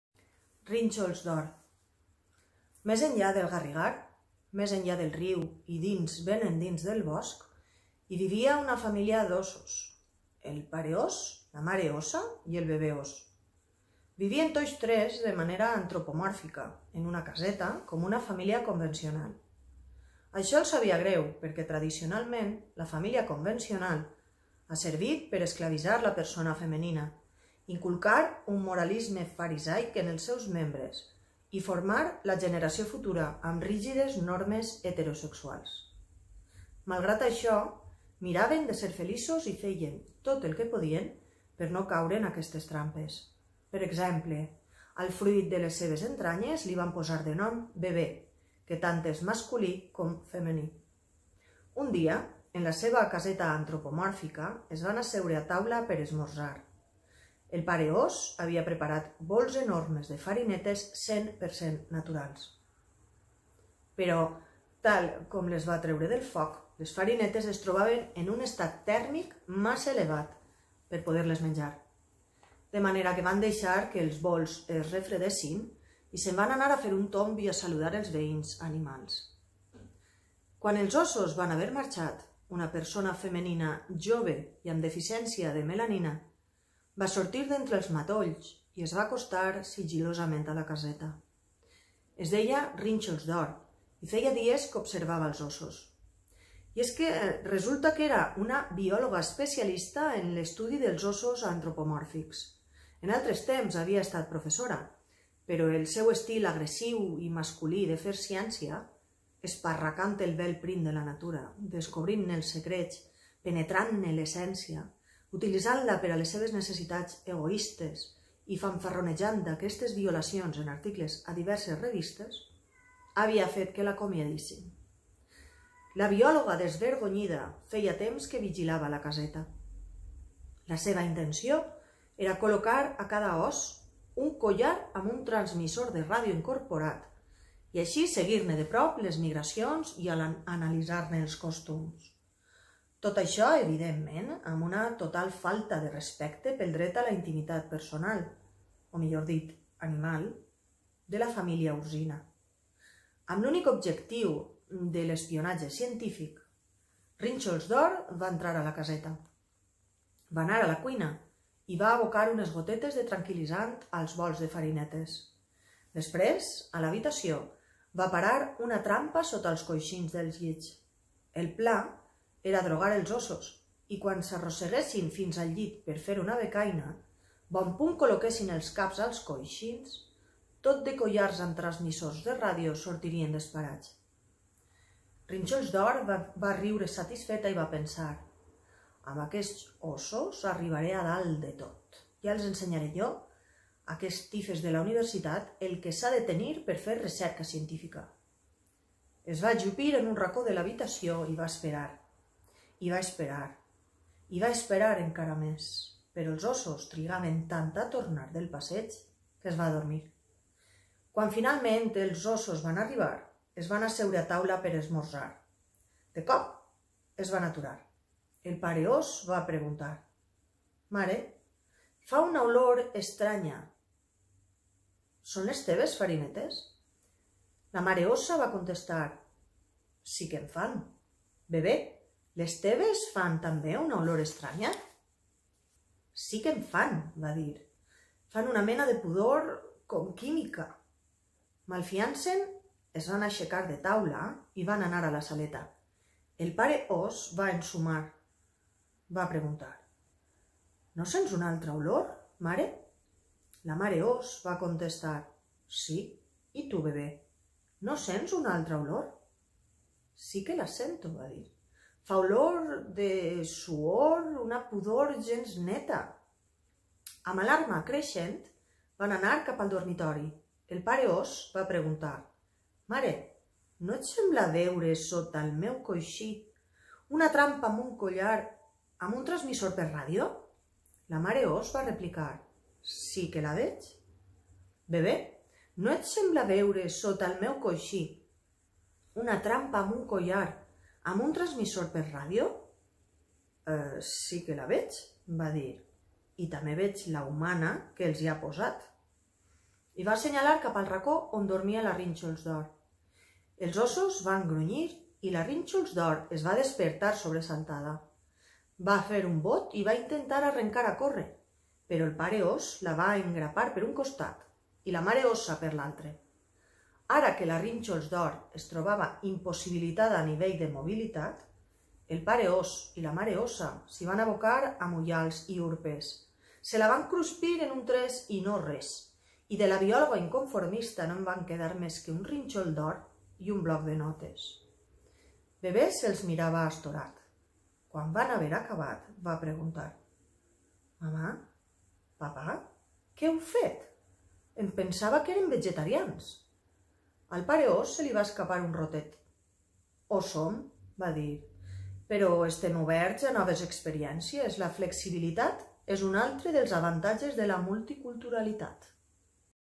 'Contes per a nens i nenes políticament correctes', "Rínxols d'or"
Conte dins de Finn Garner, James: 'Contes per a nens i nenes políticament correctes', Quaderns Crema , 1995. Traducció a càrrec de Quim Monzó i Maria Roura, 1995.